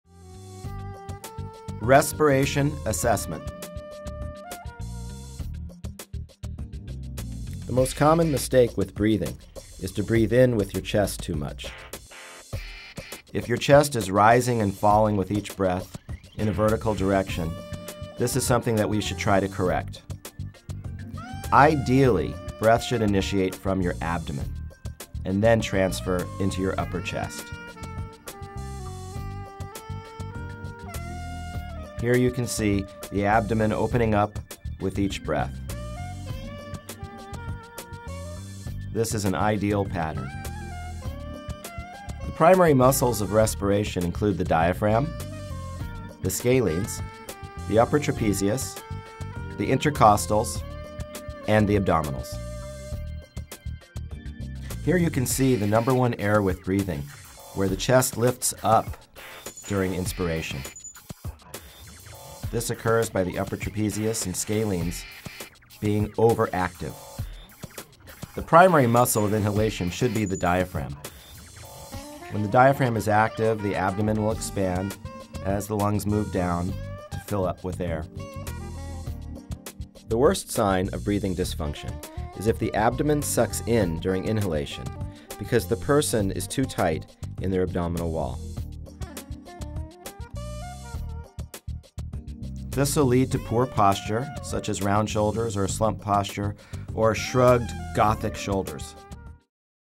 Breathing Assessment
Taken from Flexibility, Yoga Training, and Ergonomic Postural Advice DVD, this clip illustrates the common mistakes with breathing and how to correct these errors.  This clip covers the primary muscles of respiration, including the Diaphragm, Scalenes, Upper Trapezius, Intercostals, and Abdominals.